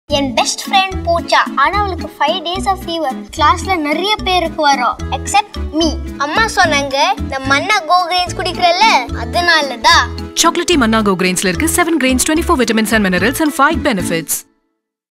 Chocolate Multigrain Drink - Manna Go Grains (15 sec Tamil MGG Immunity)
It's chocolate flavoured, it's delicious and it's healthy too.
Made from 7 grains and enriched with 24 micronutrients - Manna Go Grains has 5 growth benefits
✔ Physical Growth
✔ Mental Development
✔ Stamina
✔ Stronger Bones
✔ Immunity